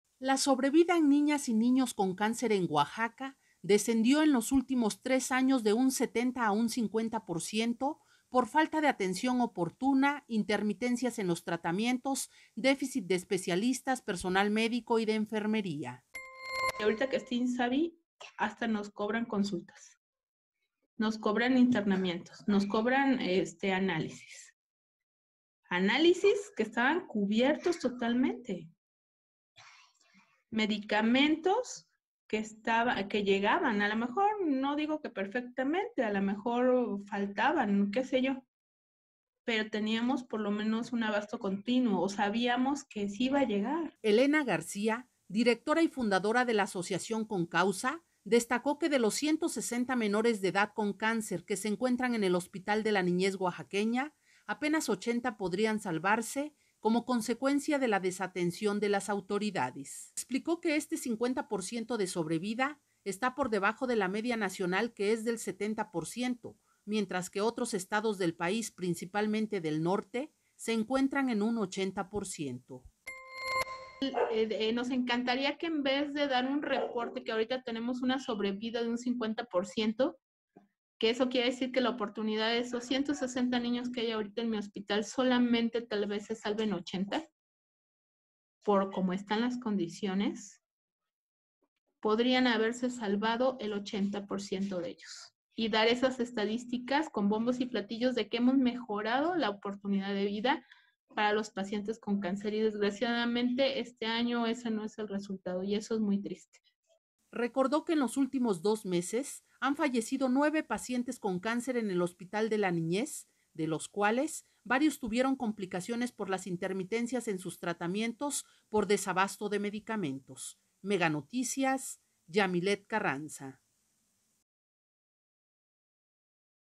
FT SOBREVIDA NIÑOS CON CÁNCER
Hasta en 20% descendió sobrevida en pacientes pediátricos con cáncer